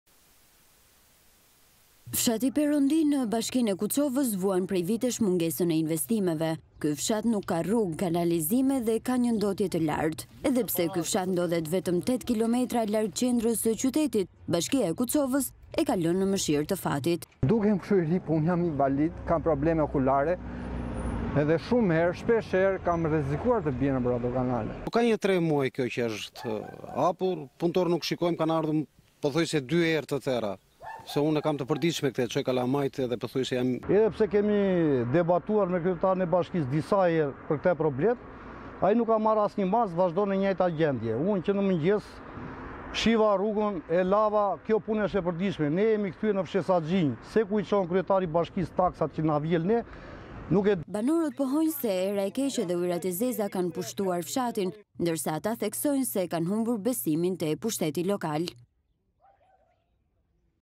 Fshati Perondi në Kuçovë pa investime, banorët: Askush nuk kujtohet për ne
Fshati Perondi në Kuçovë vuan mungesën e infrastrukturës dhe kanalizimeve. Banorët shprehen se janë lënë në harrresë nga bashkia, duke theksuar se jetesa në fshat është bërë e papërballueshme